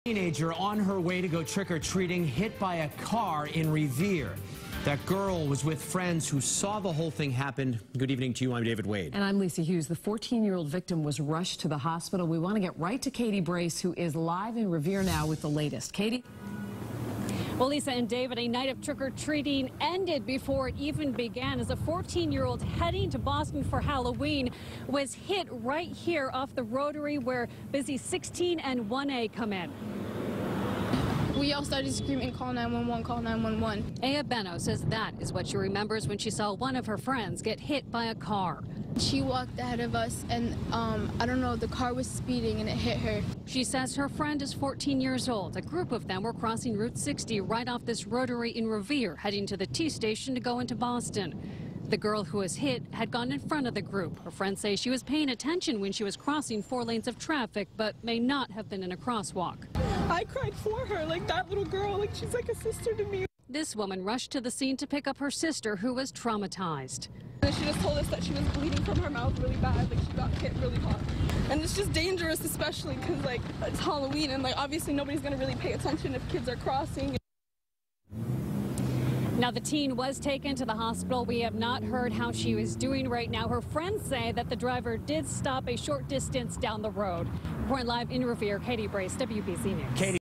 Teen Hit By Car In Revere On Her Way To Trick-Or-Treat
WBZ-TV's Katie Brace reports.